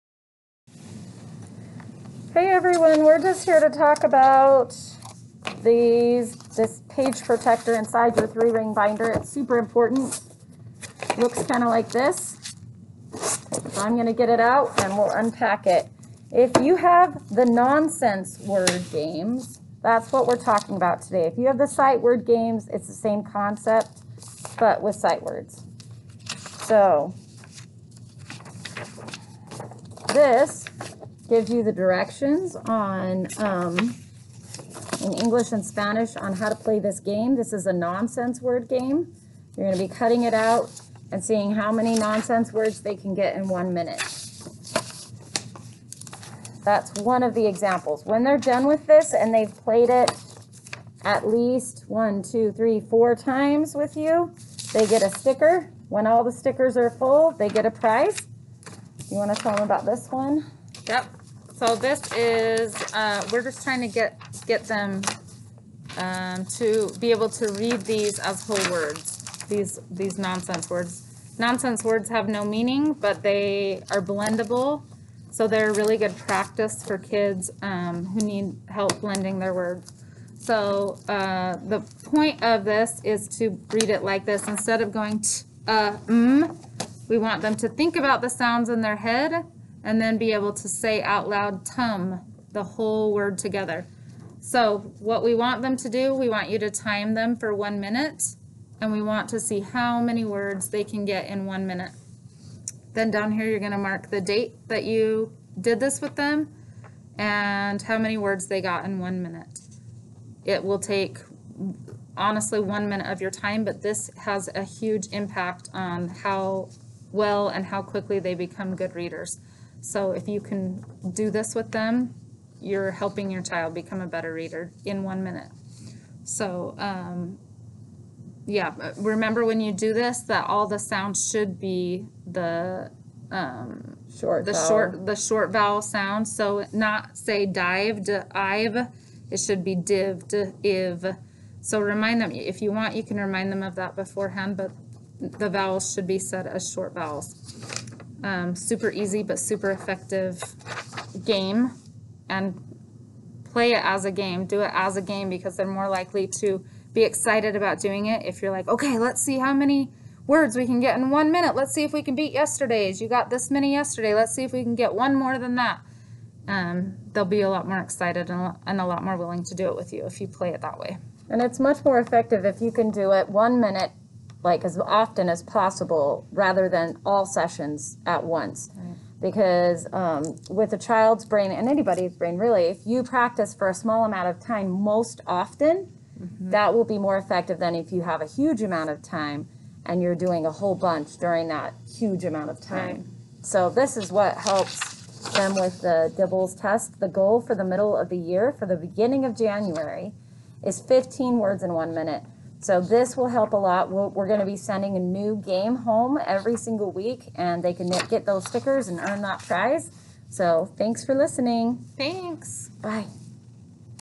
Nonsense Word Activities
Here are nonsense word activities to help your child become a better reader.